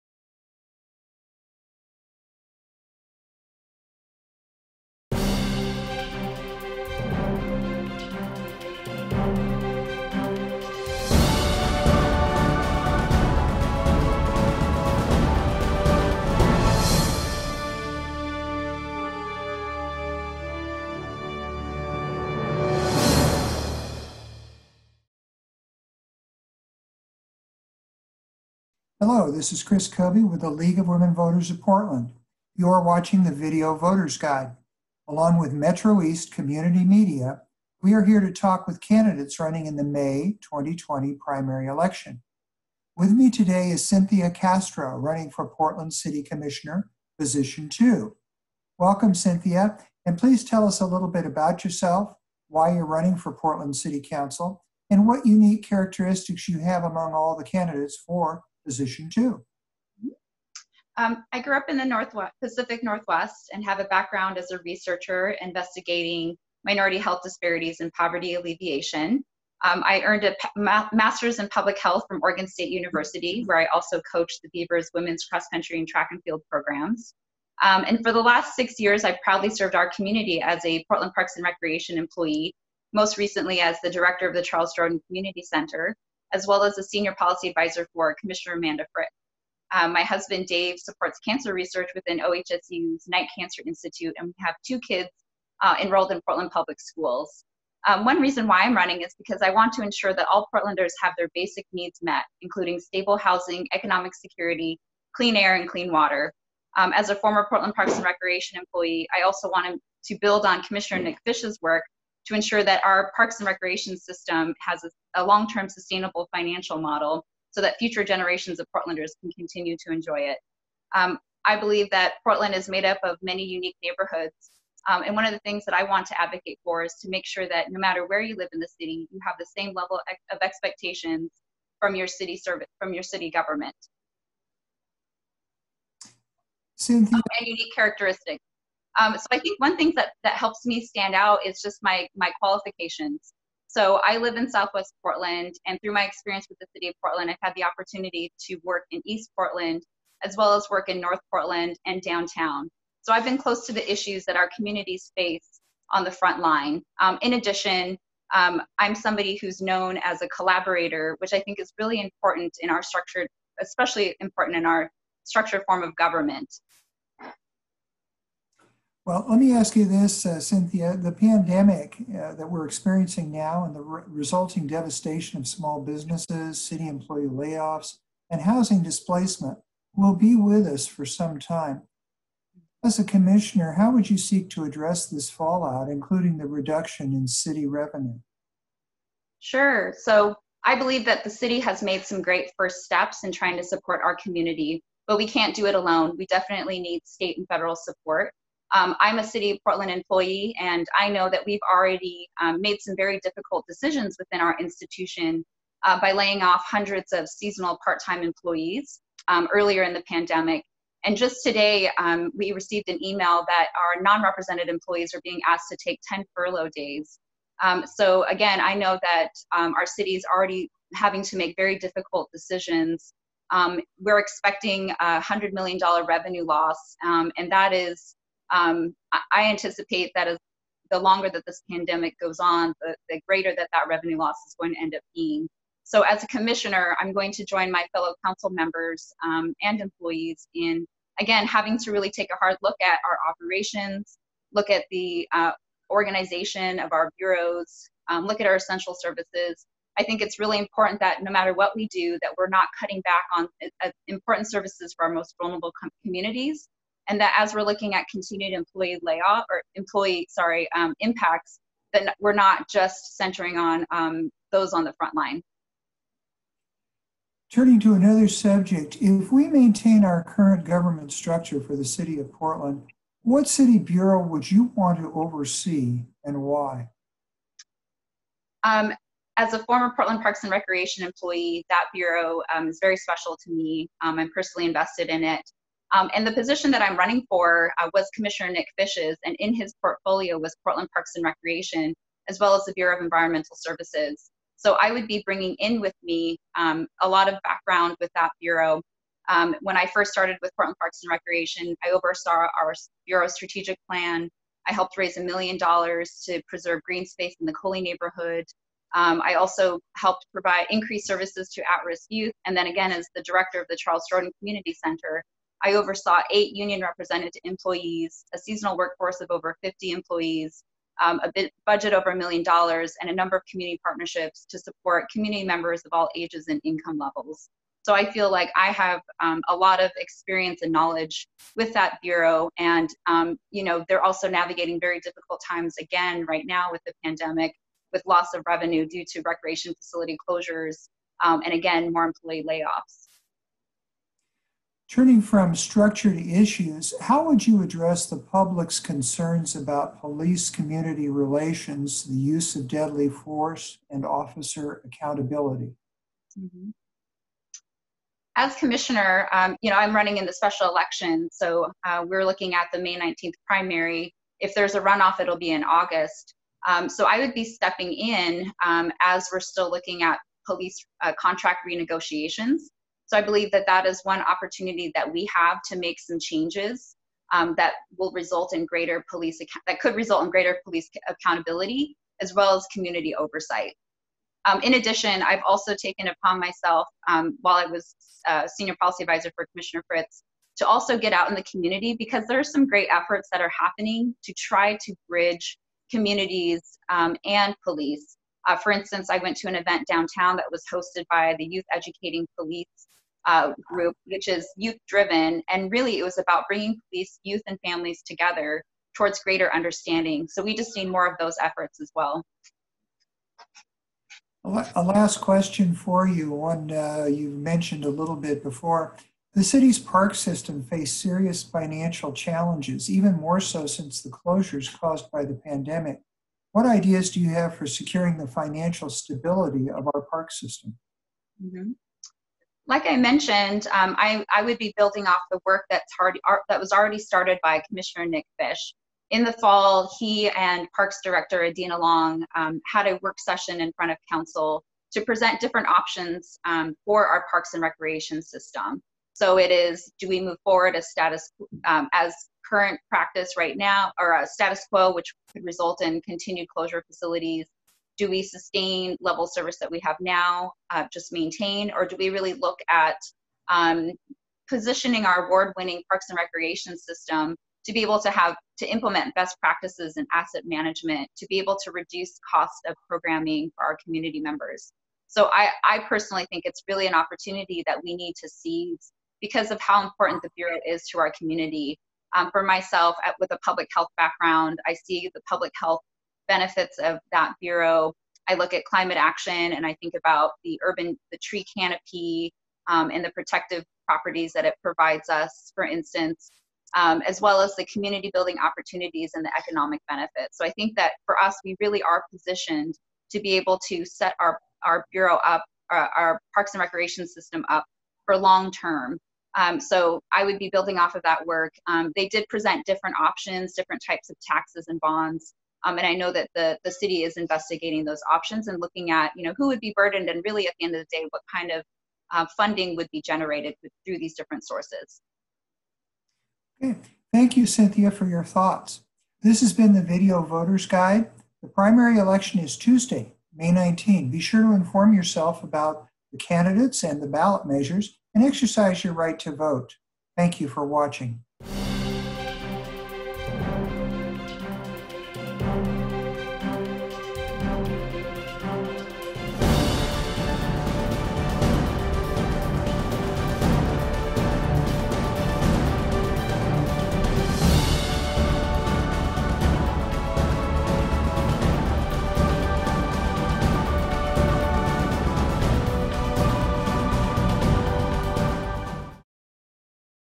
Video Voters' Guide 2020 Cynthia Castro
The League of Women Voters of Portland and MetroEast introduces you to Cynthia Castro Candidate Portland City Commissioner Position 2.